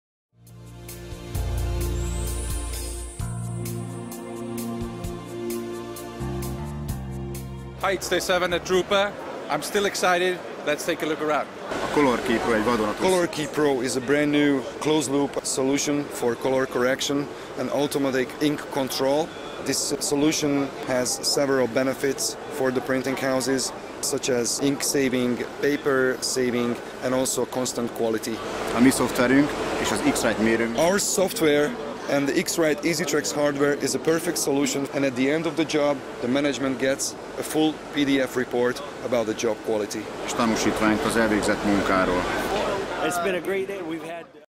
Day 7 at Drupa 2012 X-Rite - Color Key Pro
Color Key Pro's demo console equiped with X-Rite EasyTrax on Drupa 2012.